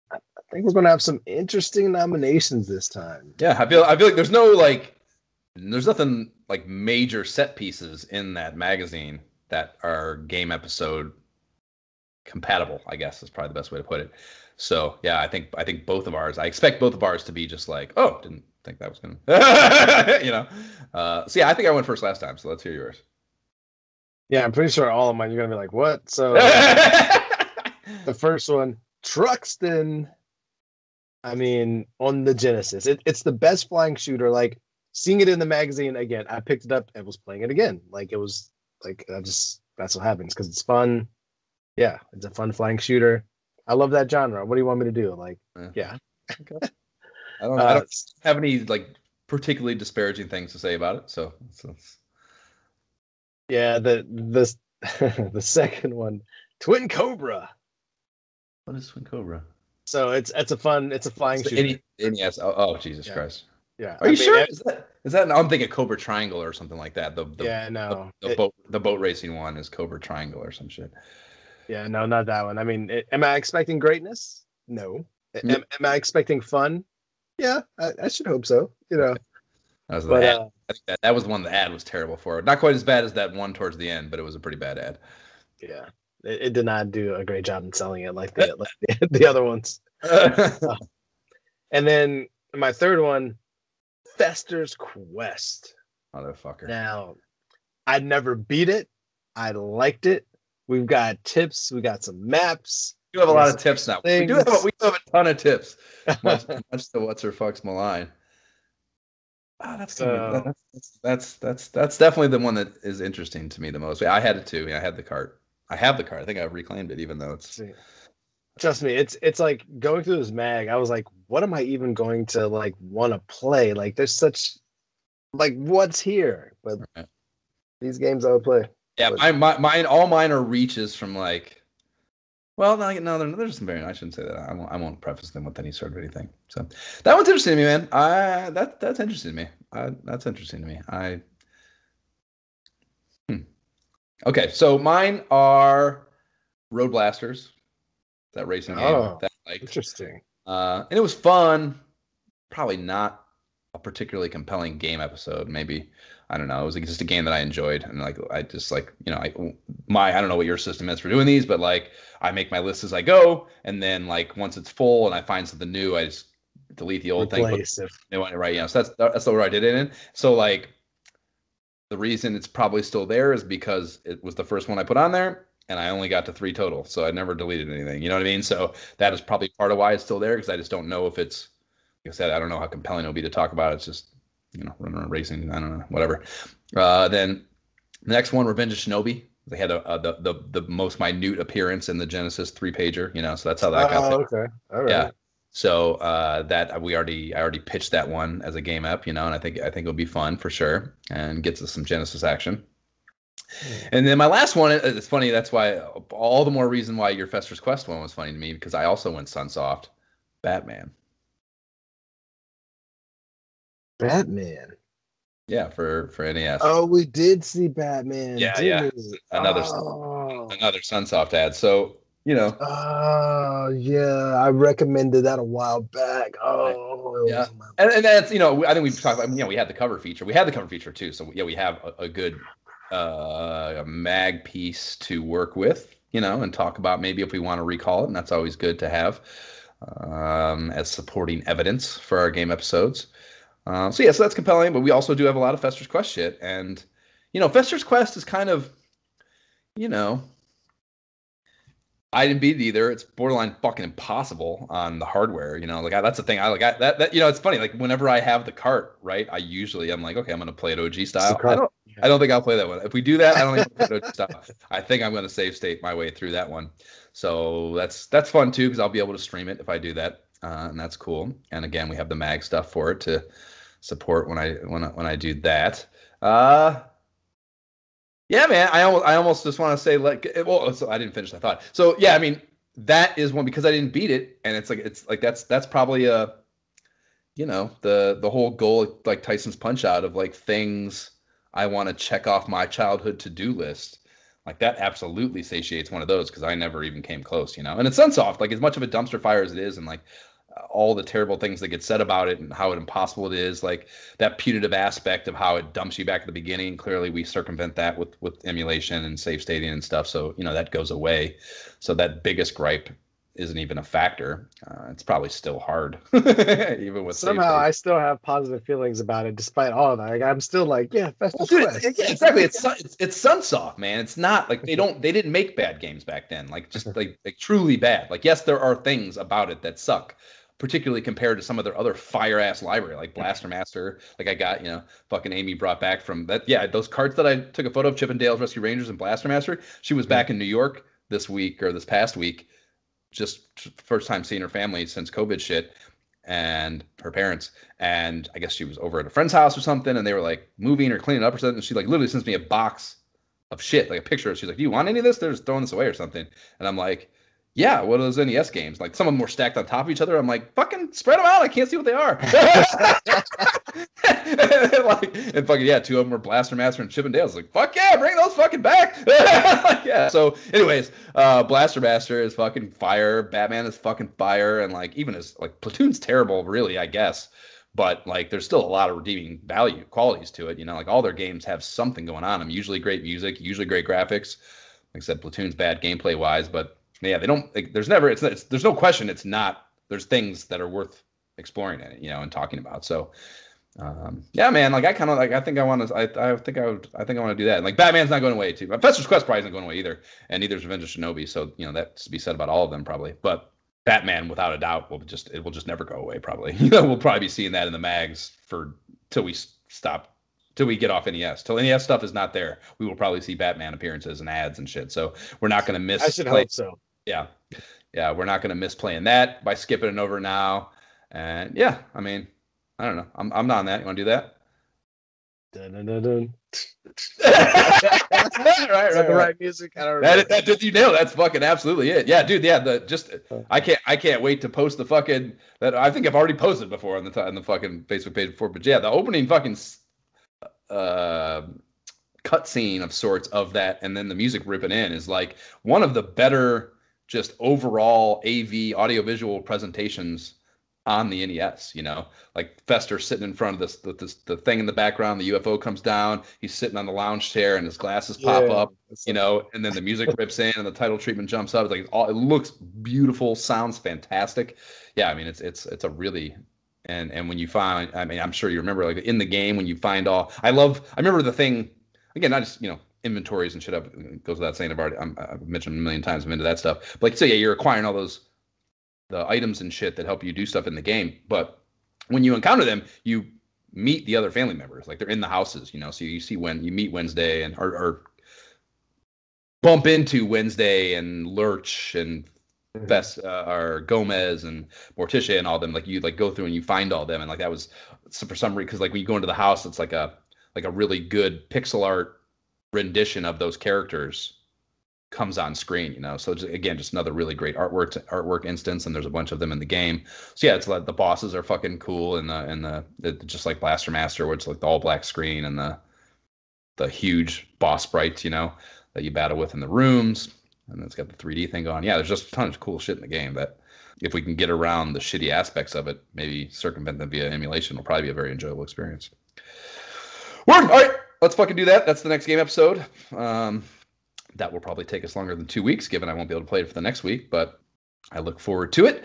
GAME EP DEBATE - The Imperial Scrolls of Honor Ep 53 (GamePro Issue #8)
Imperial Scrolls of Honor Podcast​ dudes, @gentlmanjb & @joshfolan, ponder their game episode options from GamePro issue #8 in episode 53​. retrogaming​